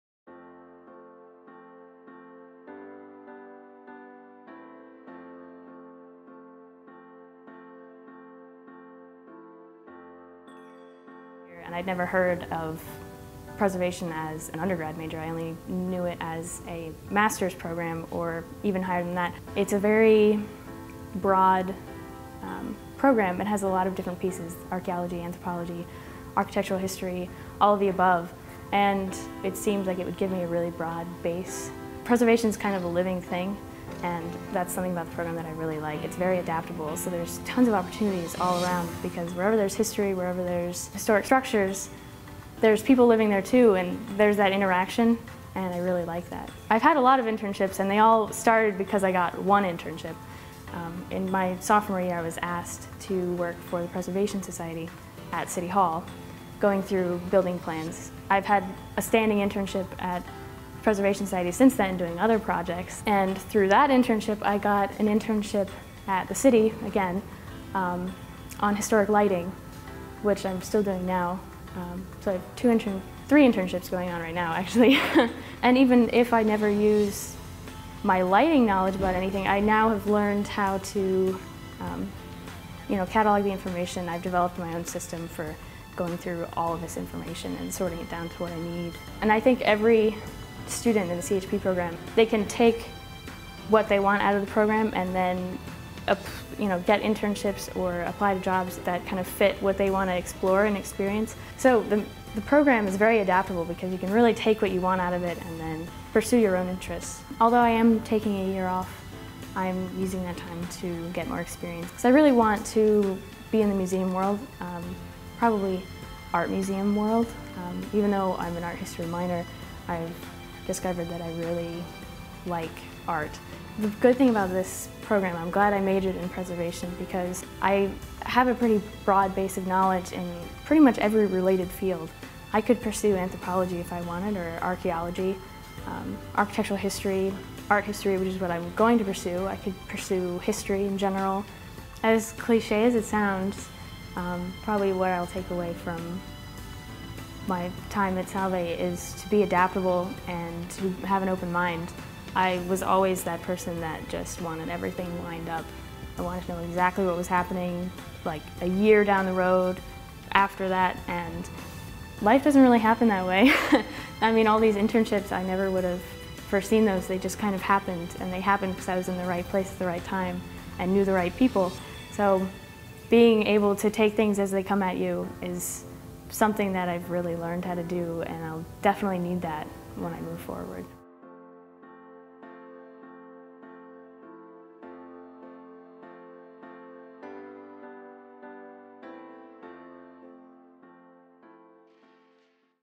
Madeline Berry '15
A broad skill set developed in the Noreen Stonor Drexel Cultural and Historic Preservation Program has equipped Madeline Berry '15 to pursue everything from archaeology to art history.